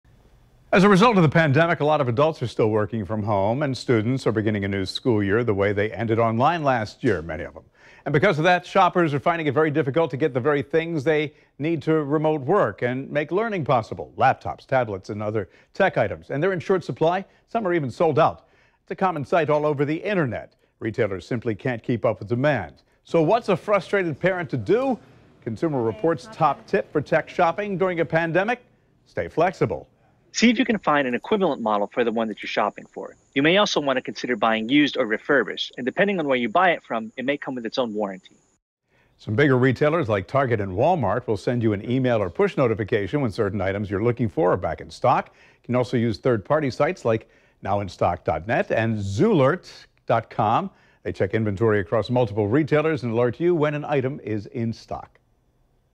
Consumer Reports: Laptop Shortage
Due to the amount of remote workers and students due COVID-19, laptops are flying off shelves faster than they can be made.